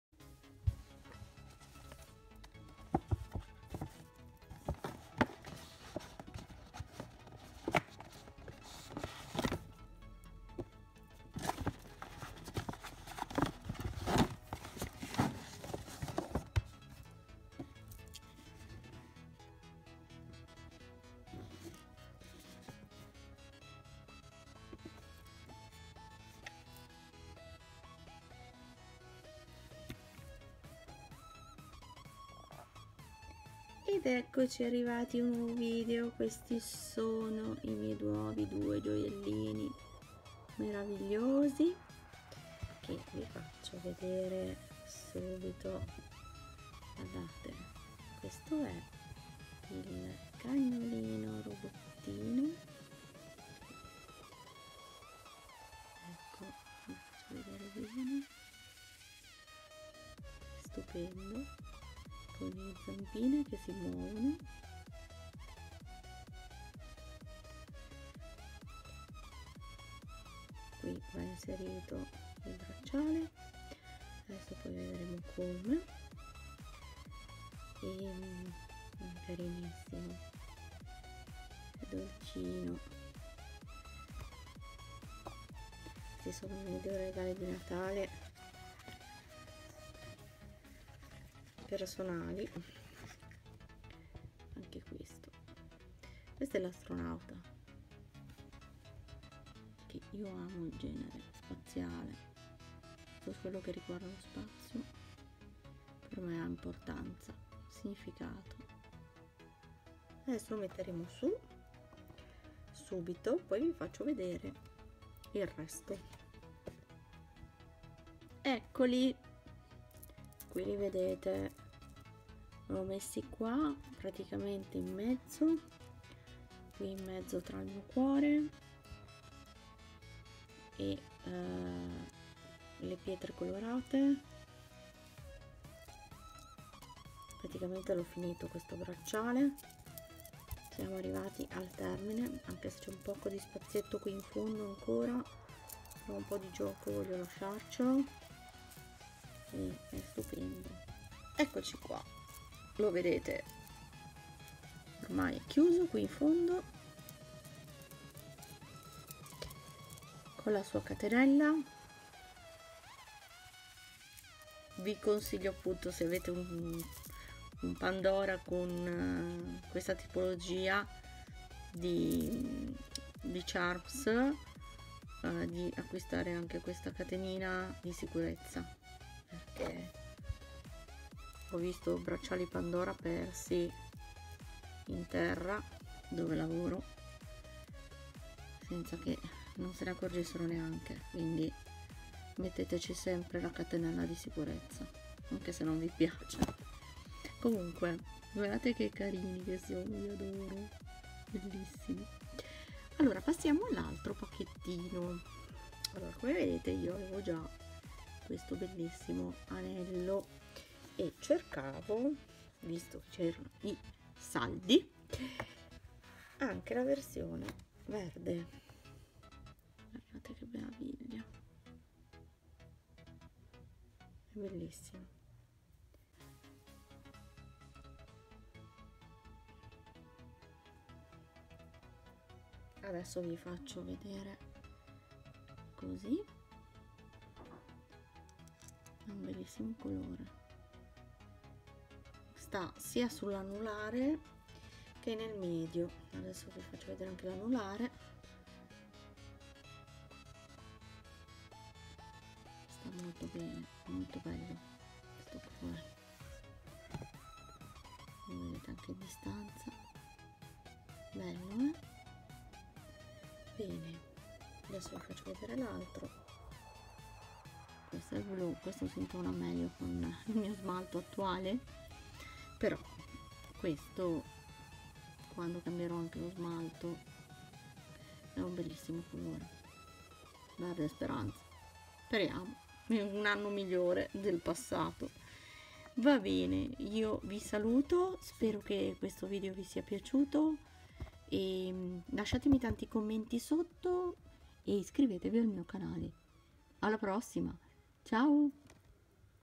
I LOVE PANDORA --- GENNAIO 2019
Nuovo video su Pandora, con i miei ultimi acquisti

il mio instagram è:   LOLLIPOP_IG

il mio Facebook è : Crylifestyle Crys